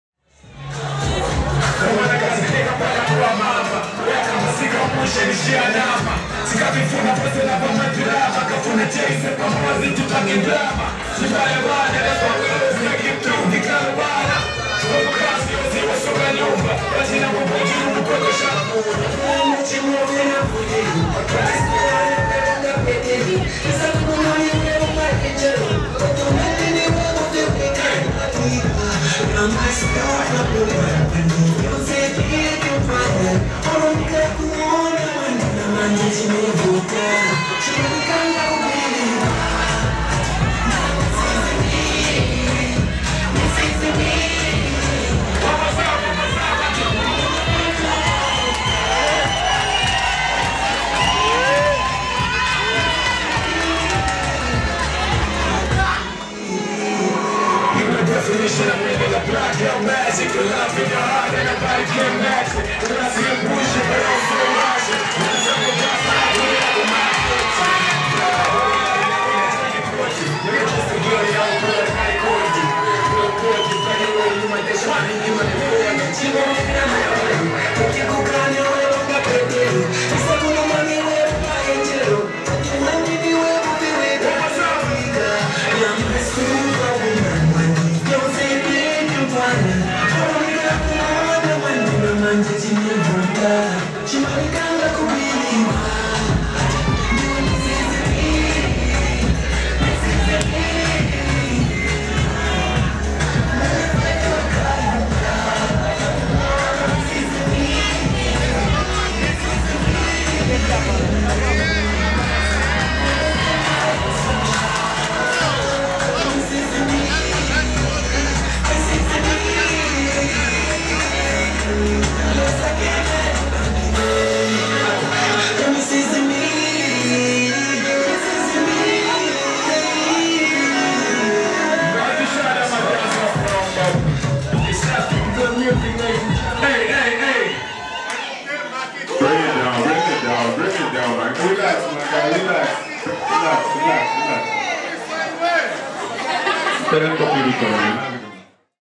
Macky 2 Made Cosmas's Father dance on the wedding day
Macky 2
Mr cosmas Ng'andwe
Wedding Day
Zambian King of Music Supports Zambia's Favorite Actor